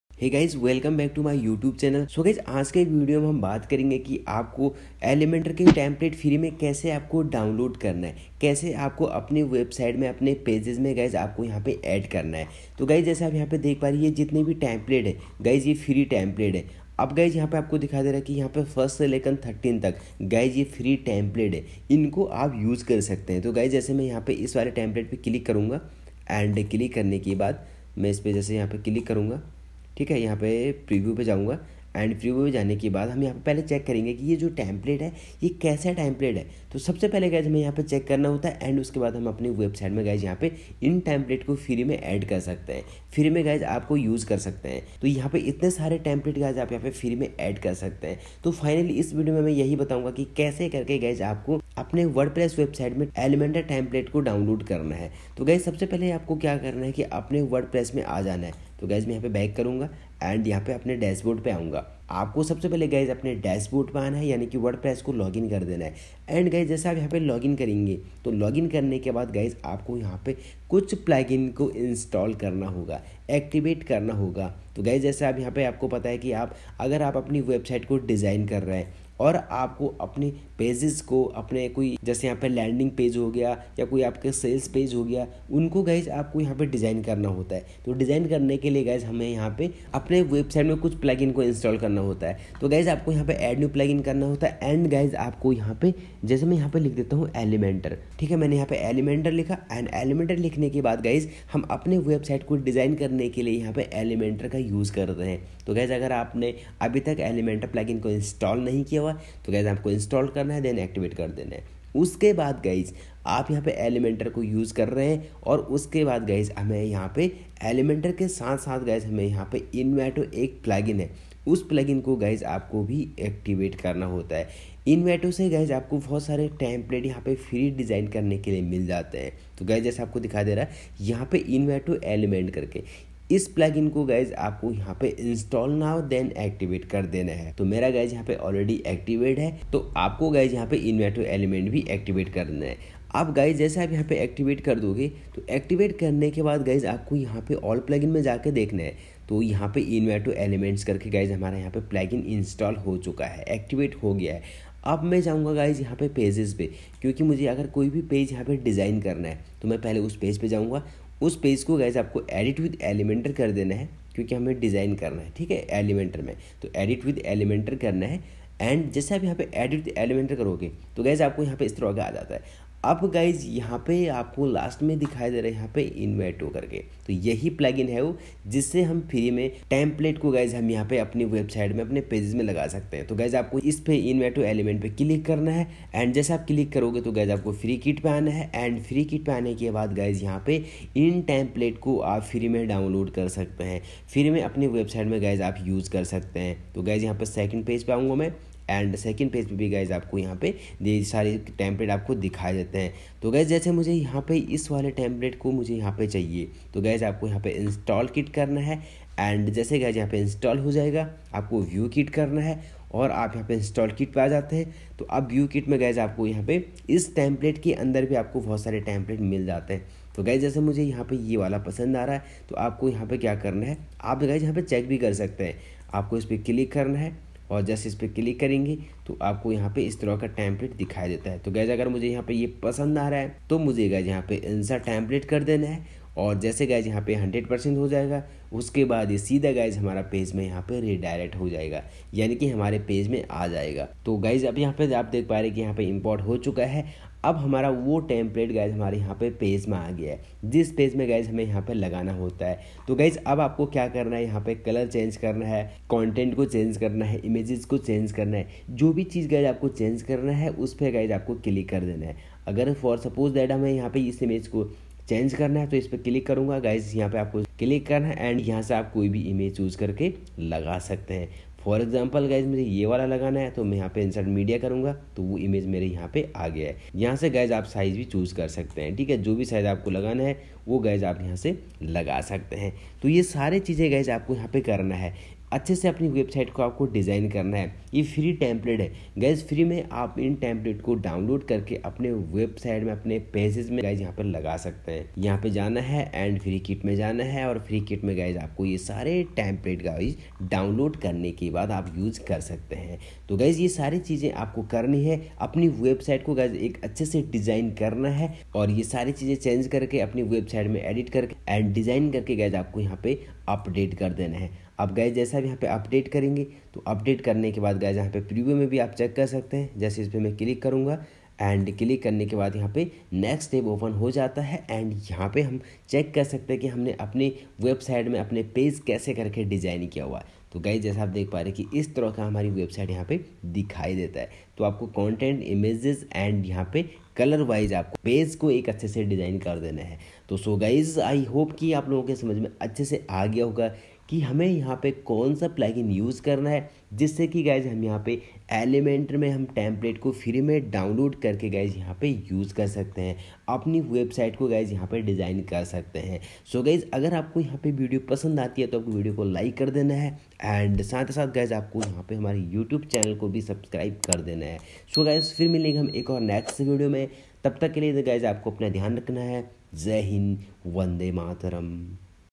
How to Download Elementor Envato Template For Free | Elementor Templates Free Download
Guy's you will learn this video How to Download Elementor Envato Template For Free | Elementor Templates Free Download or website k liye free template khan se download kre.
guy's apne wordpress website ke liye ap elementor se free me template ko download kr sakte han. bus apko apne wordpress me elementor plugin ko install krna hai us ke bad Envato plugin instal and activate krna hai fr ap apne website me pages ko template ko download kr k pages ko design kar sakte ho wo bhi free me.

Thanks